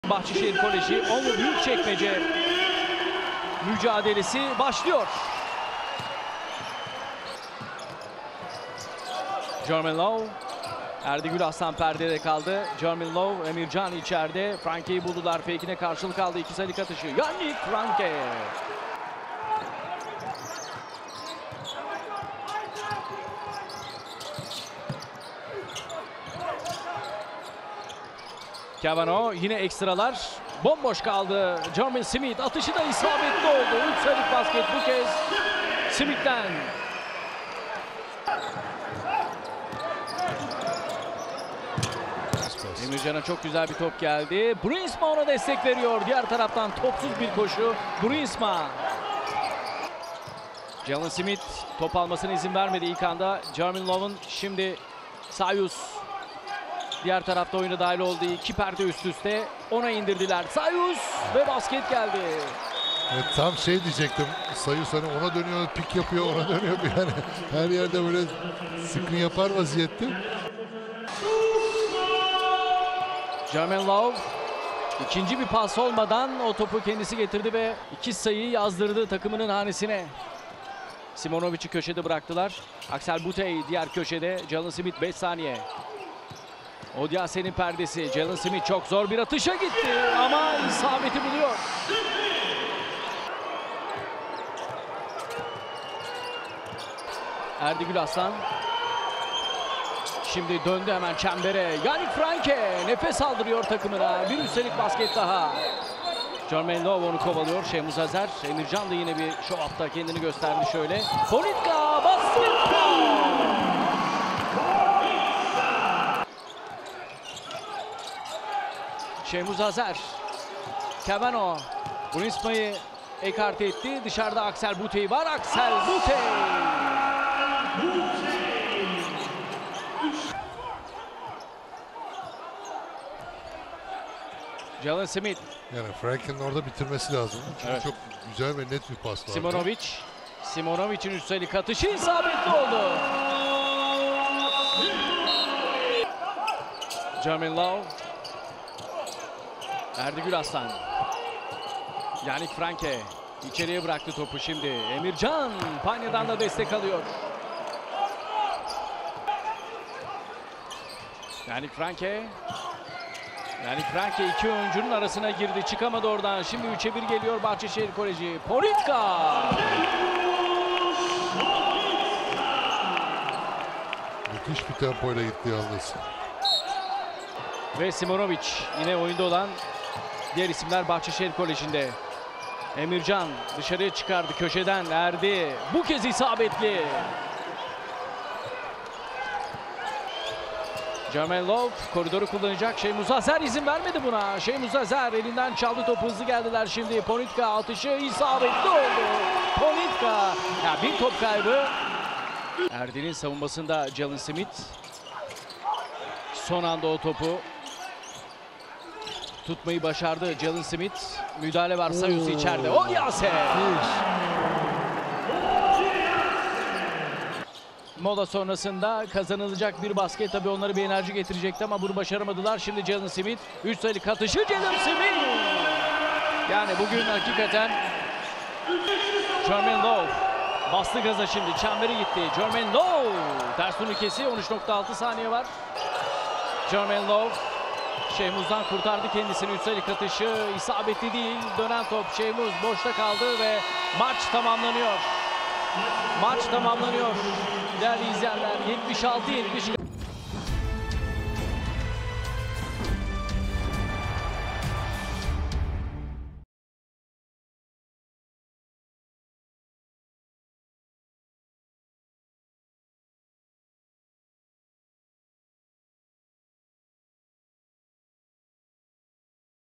Bahçeşehir Koleji (76-70) Onvo Büyükçekmece - Türkiye Sigorta Basketbol Süper Ligi - 2024/25
Türkiye Sigorta Basketbol Süper Ligi'nin 4. haftasında Bahçeşehir Koleji, konuk ettiği Onvo Büyükçekmece Basketbol'u 76-70 yendi.